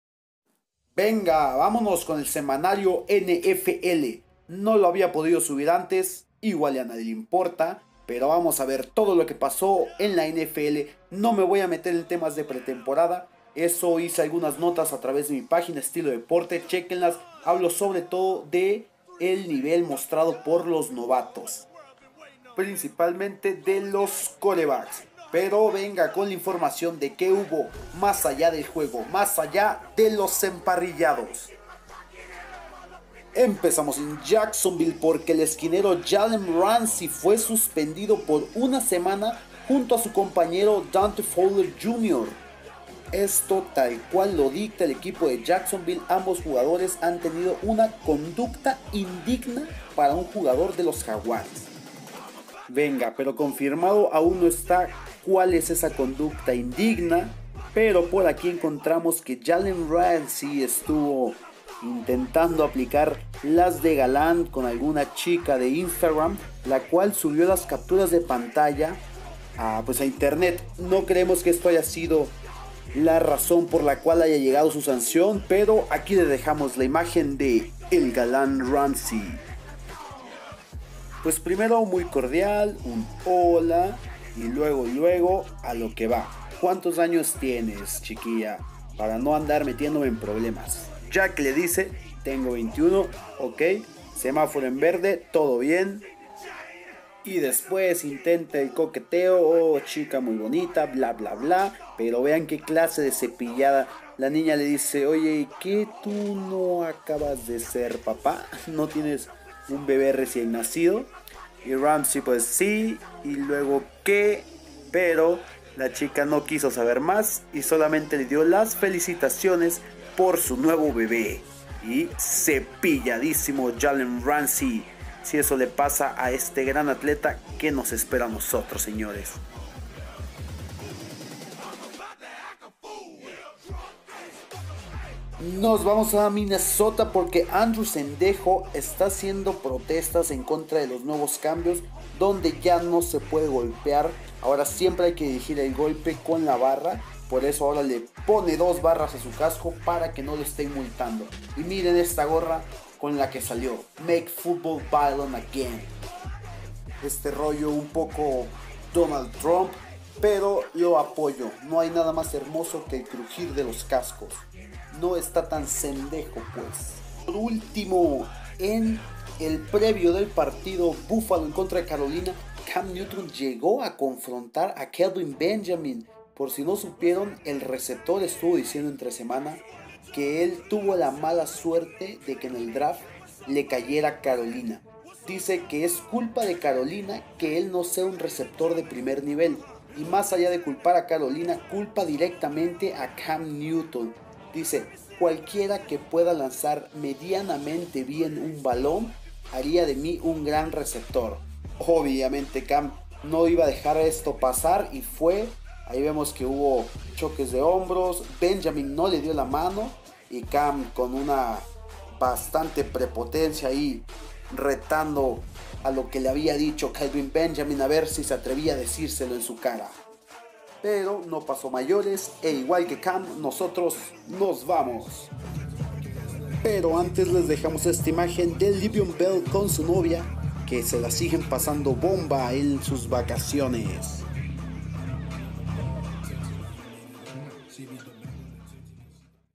SemanarioNFL Cam Newton peleando con ex compañero
Cam Newton peleando con ex compañero

Jalen Ramsey anda de galán y suspendido

Le'Veon Bell pasándola bomba en sus vacaciones